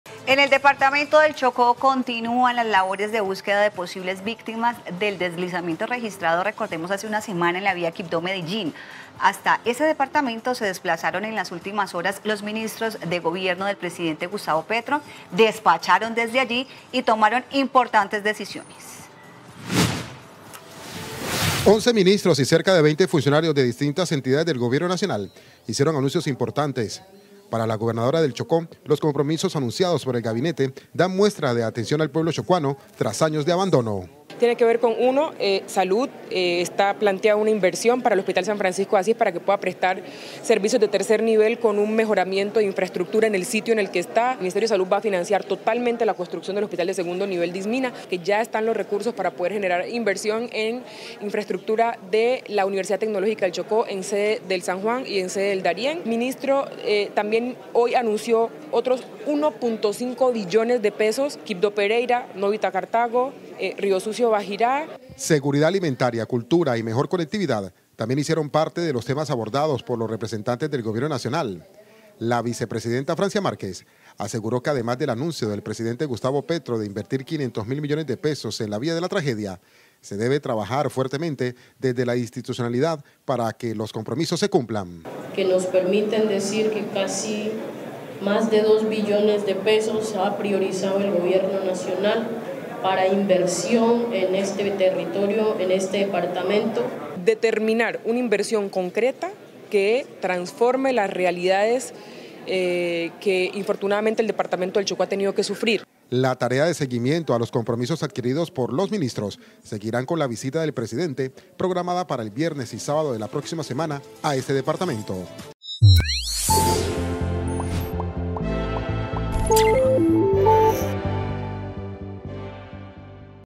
Vicepresidenta Francia Márquez y gabinete ministerial estuvieron en Chocó I19.01.2024I TP Noticias
Chocó I Continúan las labores de búsqueda de posibles víctimas del deslizamiento registrado hace una semana en la vía Quibdó- Medellín. Hasta ese departamento se desplazaron en las últimas horas los ministros del Gobierno del presidente Gustavo Petro, despacharon desde allí y tomaron importantes decisiones.